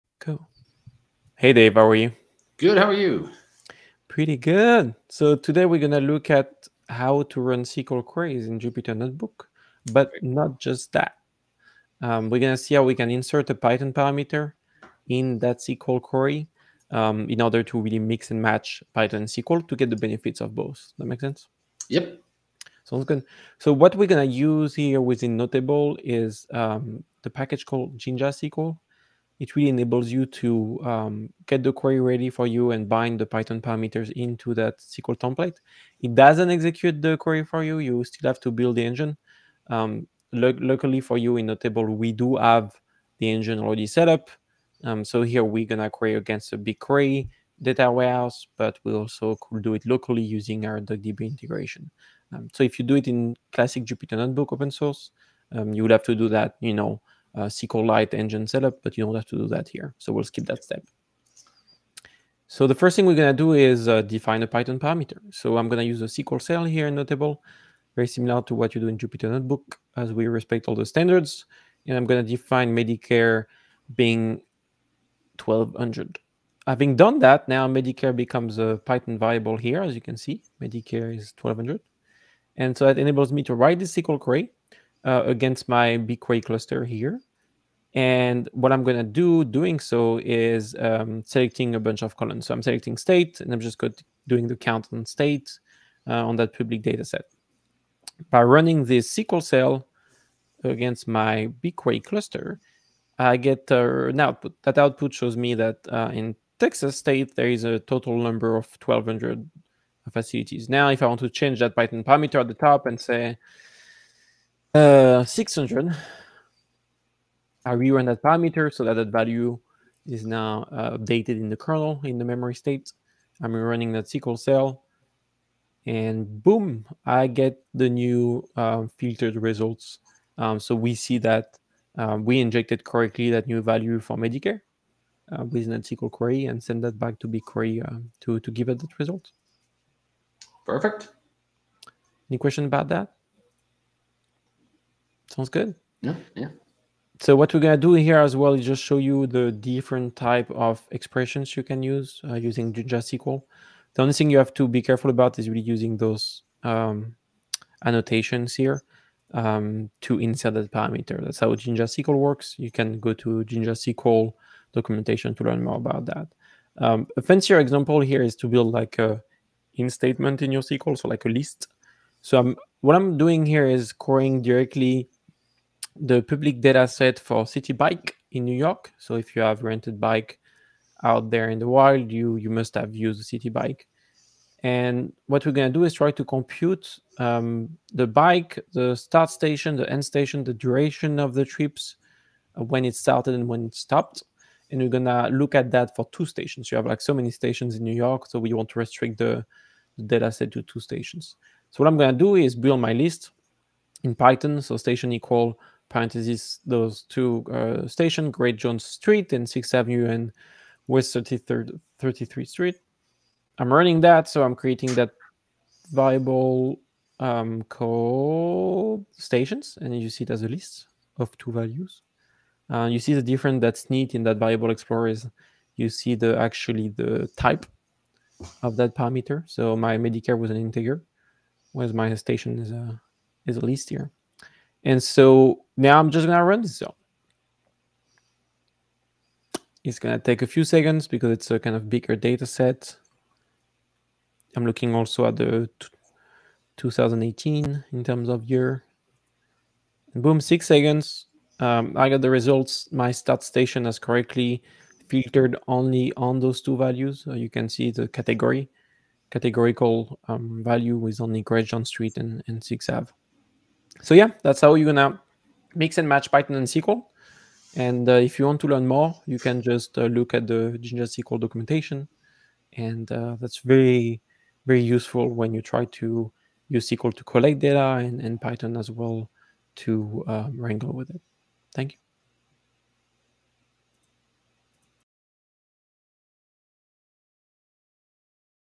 Run SQL queries in Jupyter Notebook & Insert Python Parameters using JinjaSQL within Noteable.
JinjaSQL is a Python library that allows you to write SQL queries using Jinja2 templates. It is particularly useful for Jupyter Notebook users who work with databases because it enables them to write SQL queries in a more concise and readable way.

Here are some benefits of using JinjaSQL for Jupyter Notebook:

Simplifies SQL queries: JinjaSQL allows you to use Jinja2 templates to write SQL queries. This makes the queries more concise and easier to read, reducing the chances of making syntax errors.

Better separation of concerns: JinjaSQL separates the SQL query logic from the Python code that generates the query. This makes it easier to maintain the code and makes it more modular.

Easier to parameterize queries: JinjaSQL allows you to use placeholders in your queries that can be filled in at runtime. This makes it easier to write reusable queries that can be used with different parameters.

Improved debugging: JinjaSQL allows you to use Python's debugging tools to debug your SQL queries. This can be particularly useful when working with complex queries or data transformations.

Increased productivity: By simplifying SQL queries, improving maintainability, and making debugging easier, JinjaSQL can help you work more efficiently and productively in Jupyter Notebook.